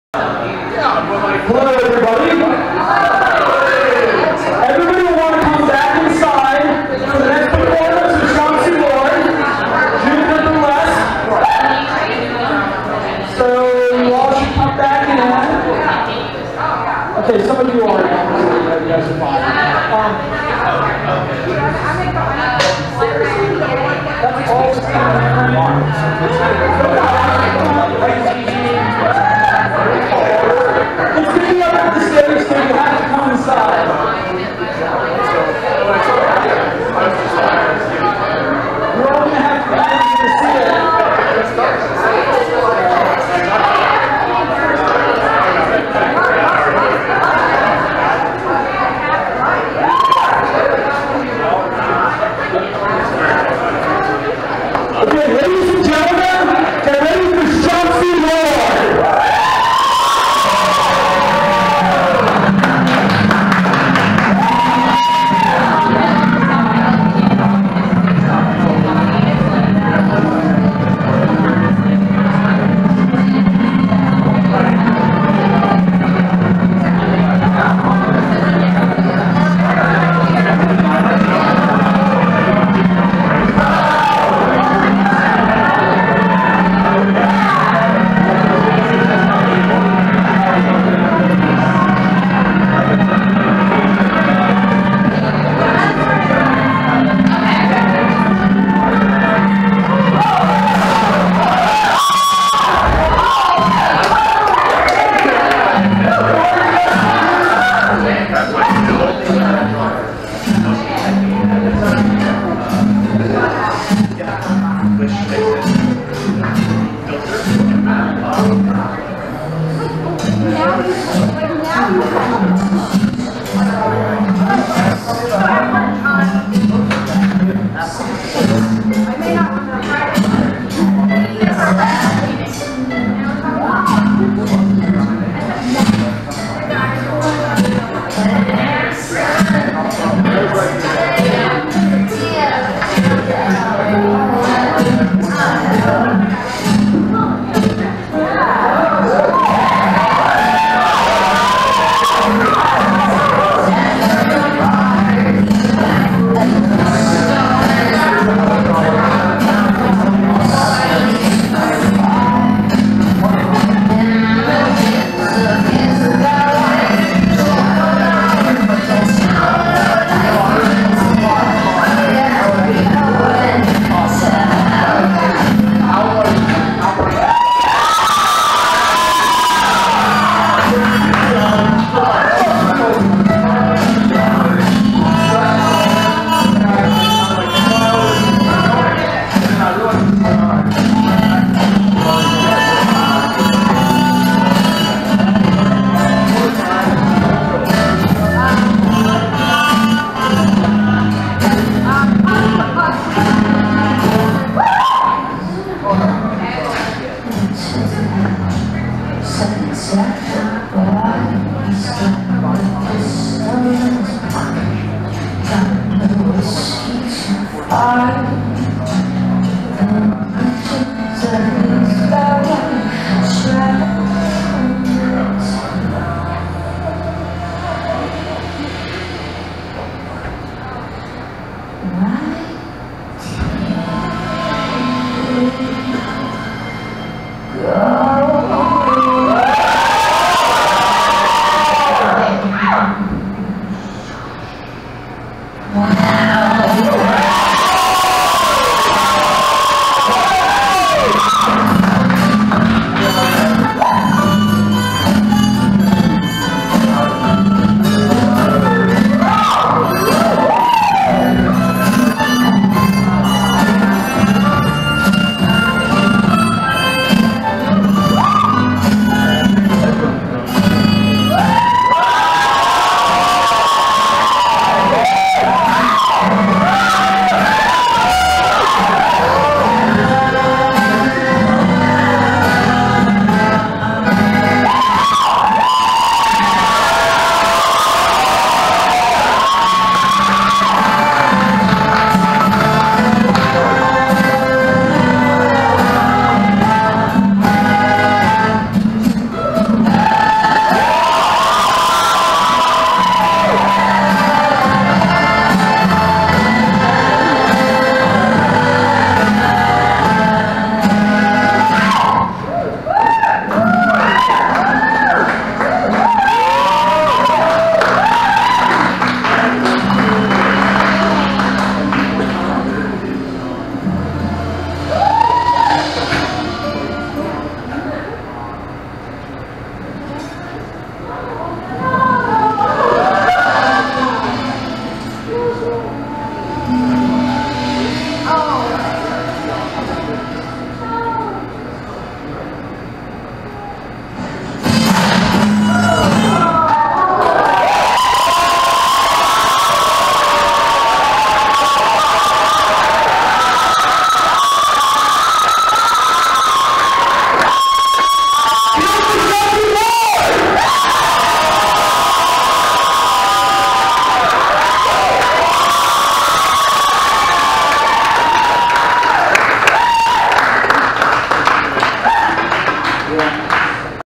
Ceiling Cam - Schatzi Lord Second Performance at Ye Olde Yellow Cabaret V
Ceiling Cam - Schatzi Lord Second Performance at Ye Olde Yellow Cabaret V (6/13/15)

This was a test of the ceiling cam at the Yellow Cab Building. The sound is off and focus isn't clear, but overall not bad for a first try.